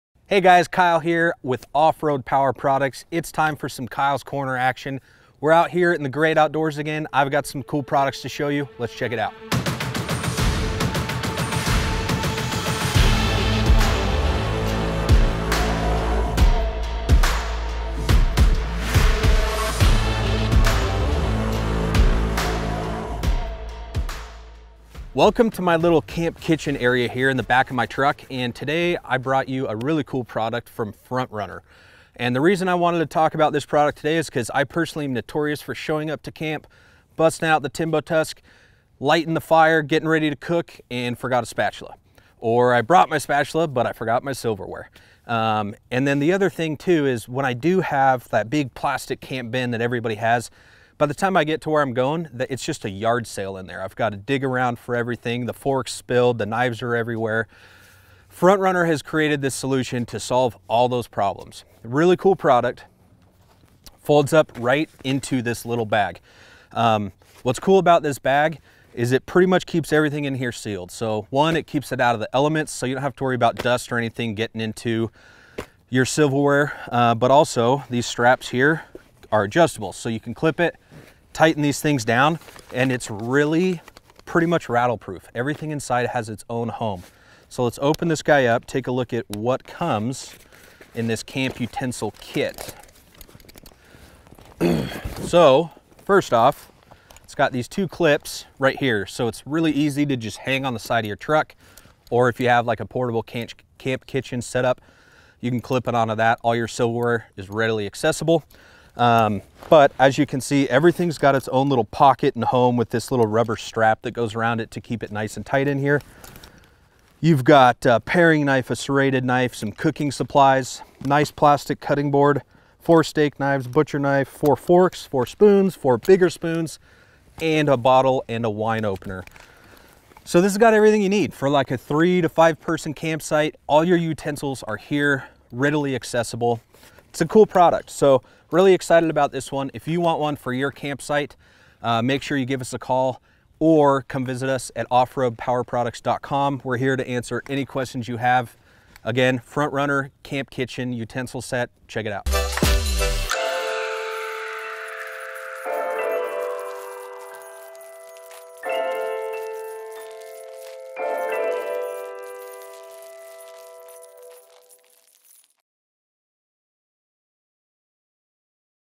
Front Runner Utensil Set makes life better
Kyle's Korner- This is a no brainer if you camp at all. All of your utensils organized, clean, and neat for cooking and eating. Front Runner really hit a homerun here.